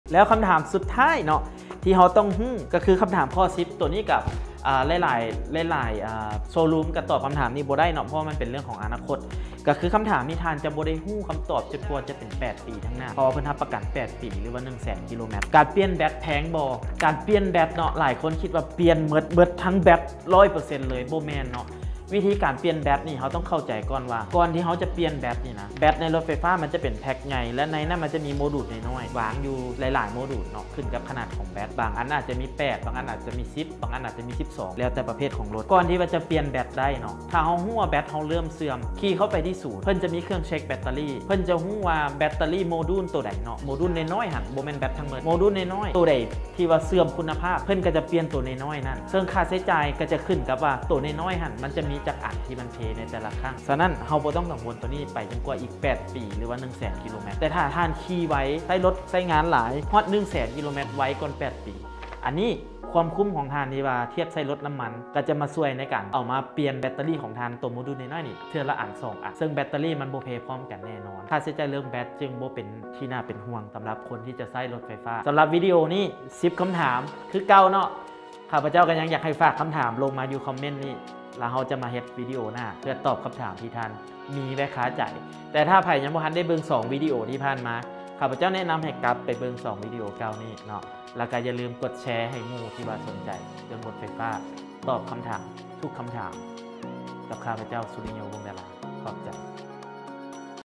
ສິ່ງທີ່ຕ້ອງຮູ້ ແລະ ຕ້ອງຖາມກ່ອນຈະຕັດສິນໃຈຊື້ລົດໄຟຟ້າ
ການປ່ຽນແບັດແພງບໍ